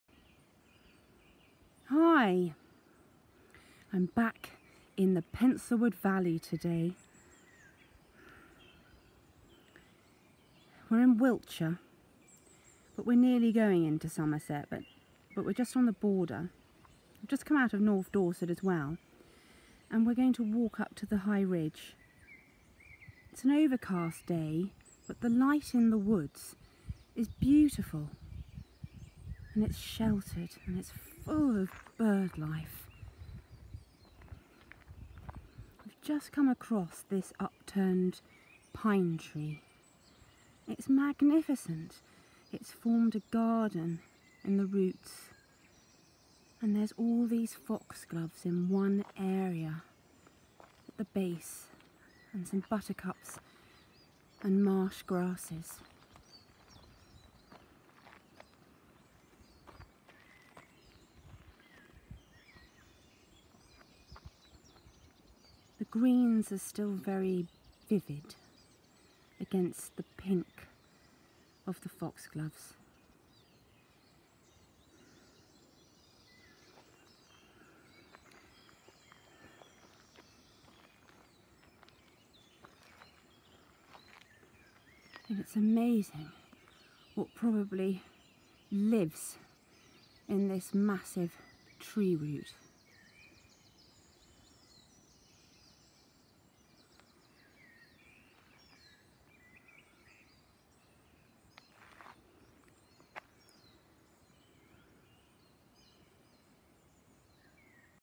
I’m loving nature in the Penslewood valley in Wiltshire by Professional Artist Samantha Adams.
Enjoying a walk in the woods in June.